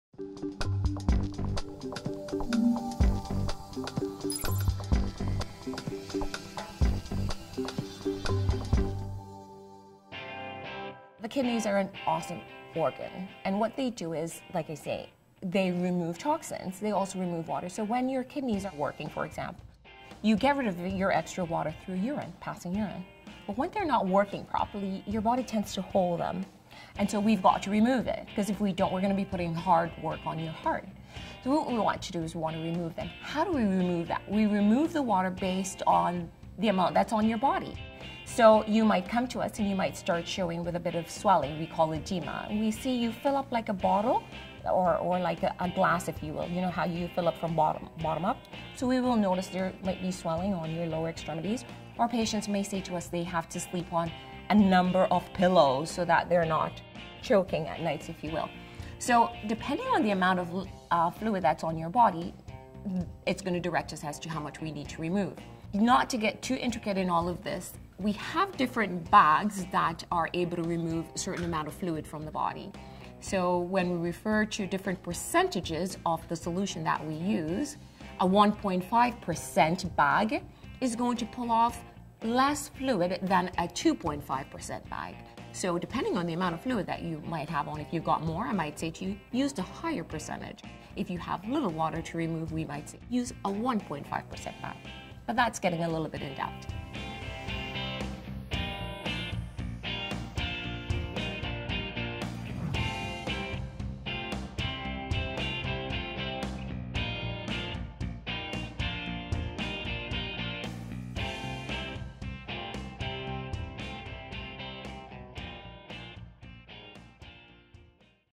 Water Retention Management
Many peritoneal dialysis patients undergo water retention management, but what is that?

The kidneys remove toxins and water from the body. When the kidneys aren't working properly, the body stores excess water, which needs to be removed.

So, how is this water removed? Watch the video to hear Carlene McLean from the Scarborough Health Network explain.

To learn more about the fight to end type 2 diabetes, go